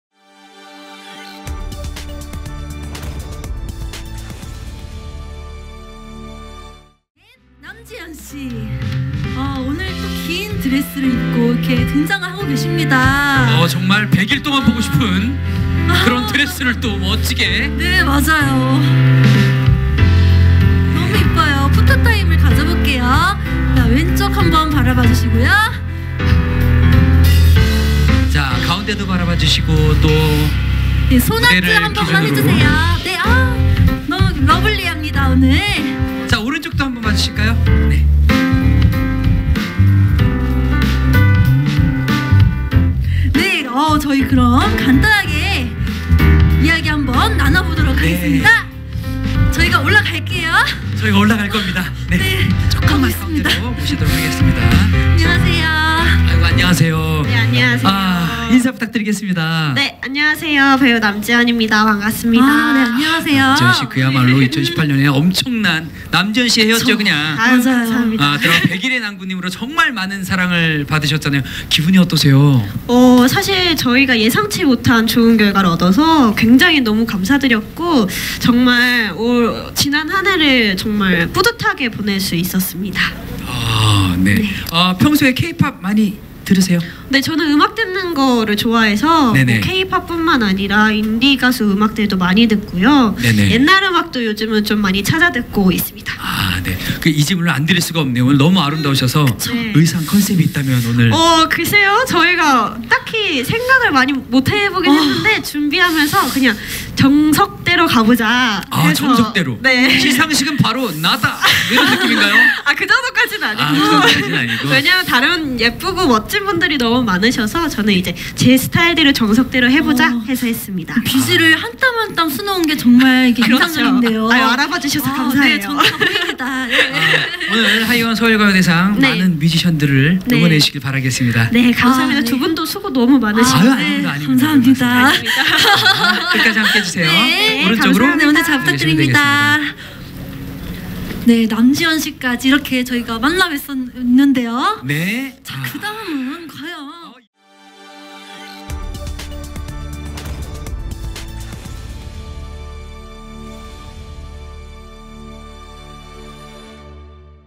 남지현, 순수함 속 넘치는 여성미 "오늘 의상은요…" ('제28회 서울가요대상 레드카펫')
[스포츠서울 박경호기자] '제28회 하이원 서울가요대상' 시상식이 15일 오후 서울 구로구 고척스카이돔에서 열렸다.

한편, 2019 서울가요대상은 방탄소년단이 대상을 차지했다.

사진 | 스포츠서울DB